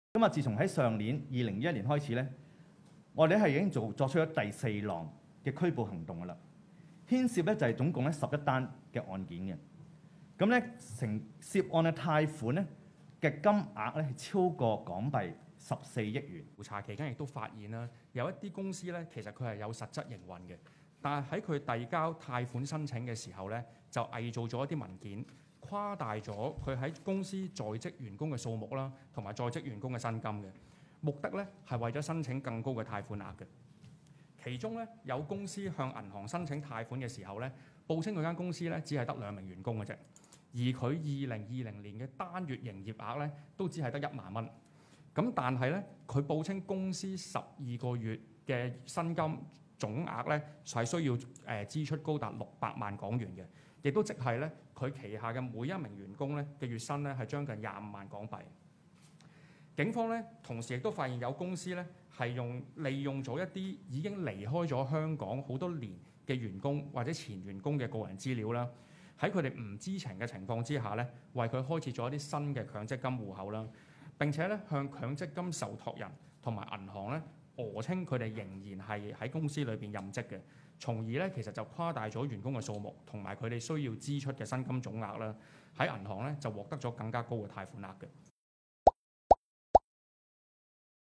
18人詐騙「百分百擔保特惠貸款」被捕
警方發現有詐騙集團涉嫌利用虛假文件，虛報公司營運及僱員開支，詐騙政府推出的「百分百擔保特惠貸款」。經調查後於7月下旬採取「擊築」行動，拘捕12男6女，涉嫌「串謀詐騙」及「洗黑錢」，涉案金額達1.93億元，並凍結2,200萬元資產。